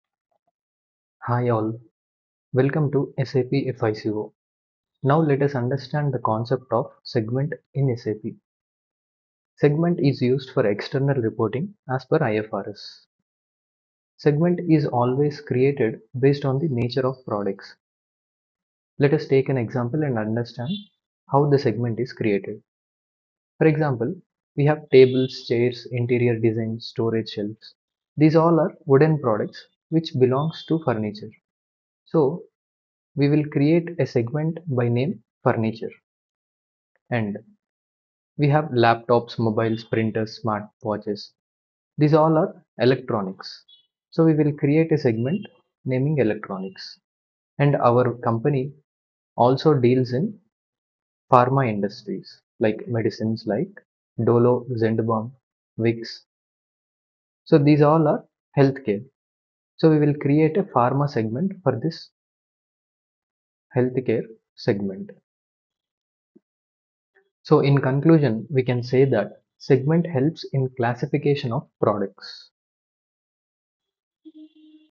Segment in sapfico
In this video we will know the concept of Segment in SAP FICO with examples